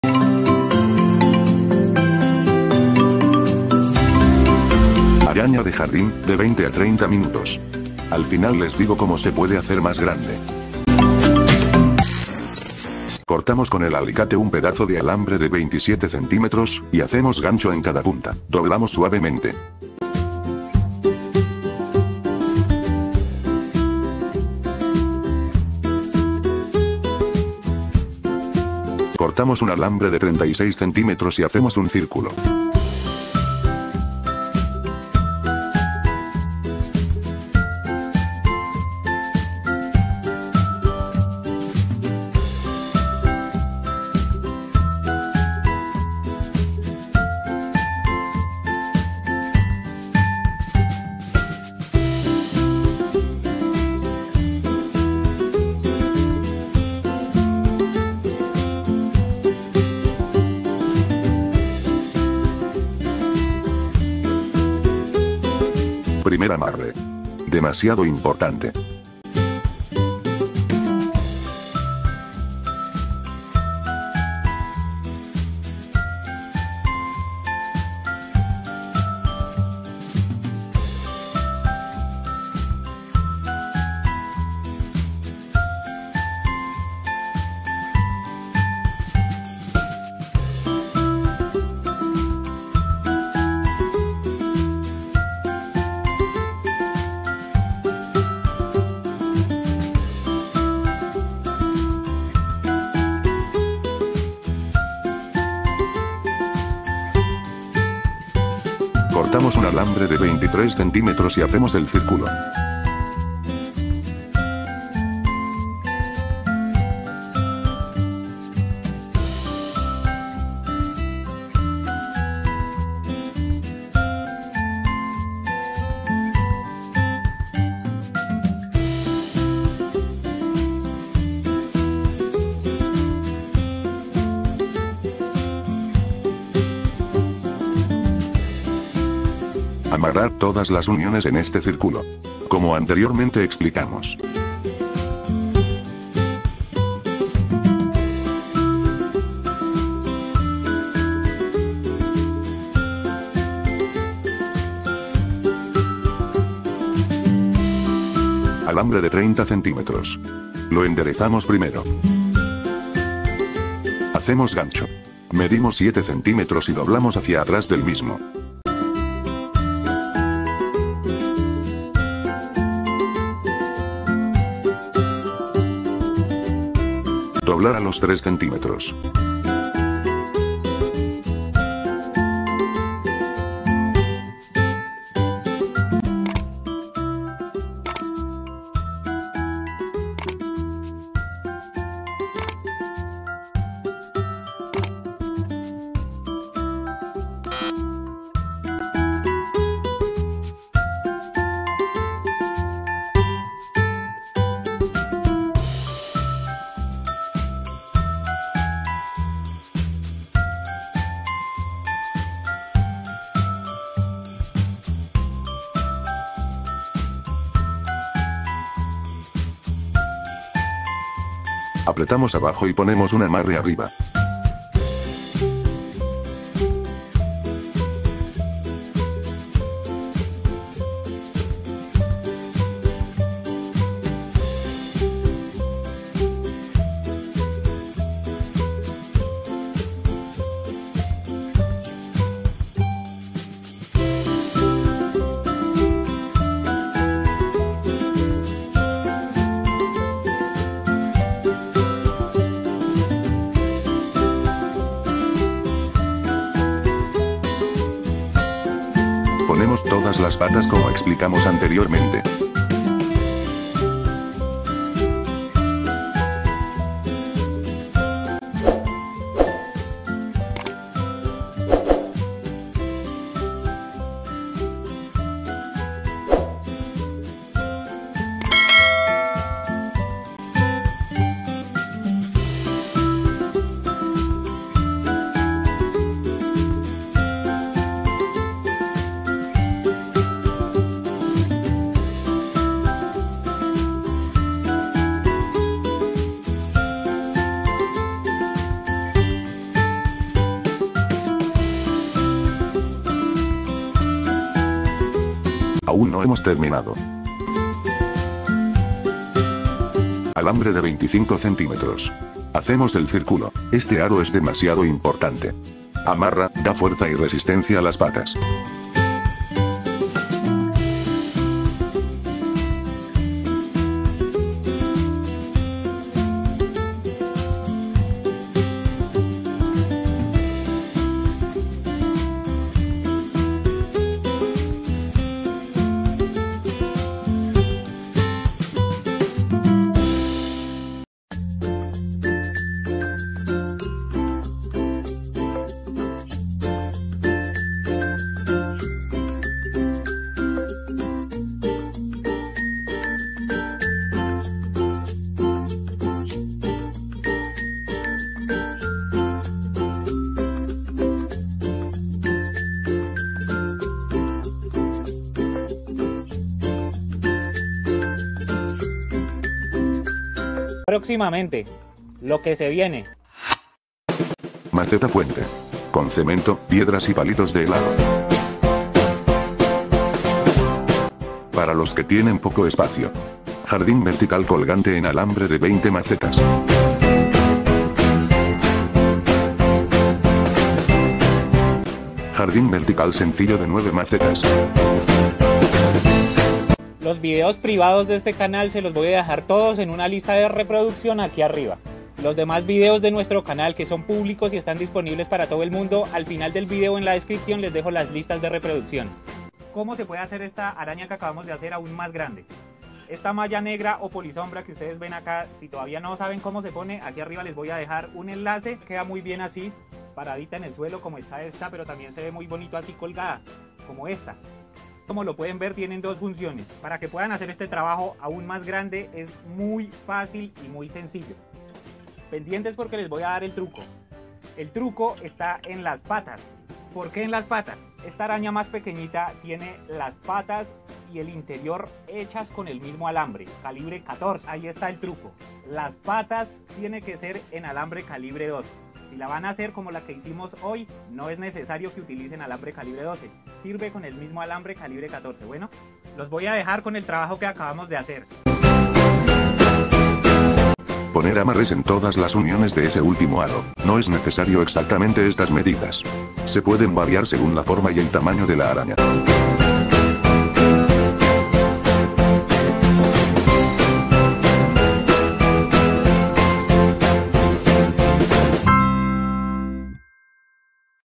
ARAÑA para plantas [porta maceta] Idea de jardin de doble uso con Alambre
en este vídeo verás como se hace  una bonita araña para el jardín de doble uso ¡de colgar o de poner en el piso! además sin soldar como siempre sólo con amarres pero muy resistente y fuerte, hace parte del jardín colgante de 20 macetas que se enseña a hacer en ARTEJARDIN, les dejaré las listas de reproducción con todos los vídeos tutoriales por si no se han visto alguno.